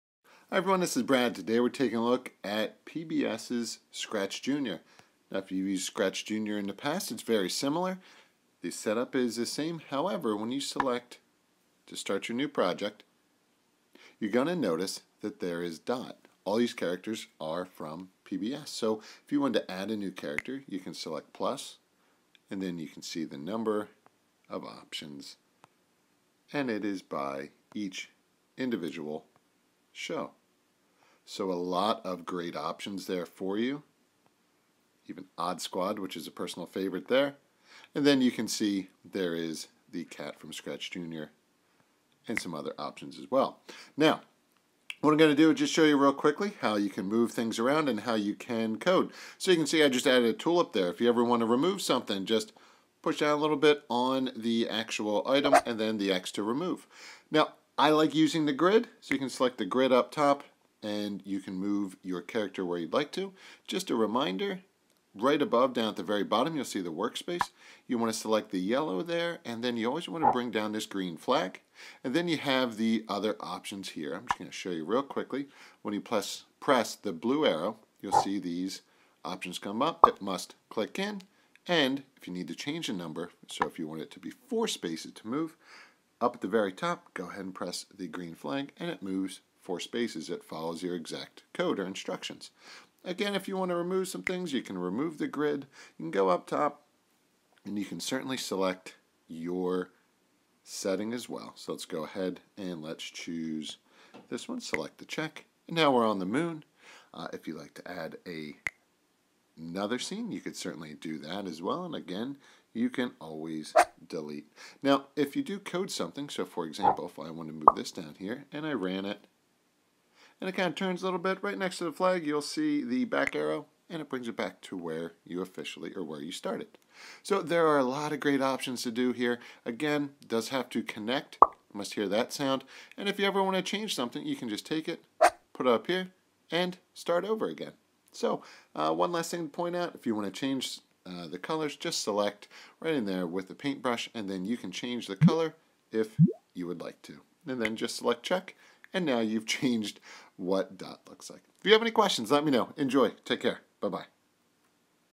PBS Scratch Jr. - learn programming based on PBS characters!
If you have used Scratch Jr you will notice the similarities between the two apps. The basic difference is the availability of PBS themed characters available for students to use in their "story"!